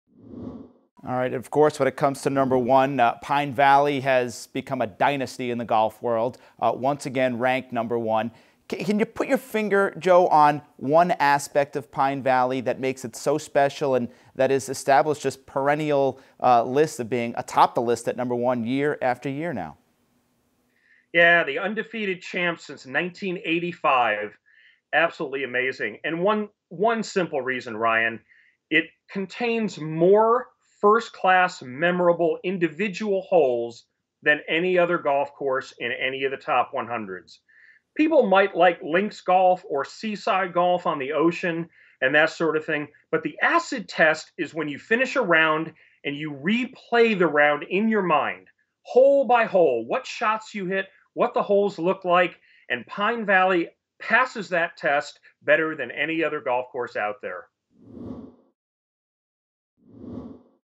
Pine Valley: Undefeated since 1985 | GOLF.com
Travelin' Joe Passov explains why Pine Valley is the No. 1 course in the world, year after year.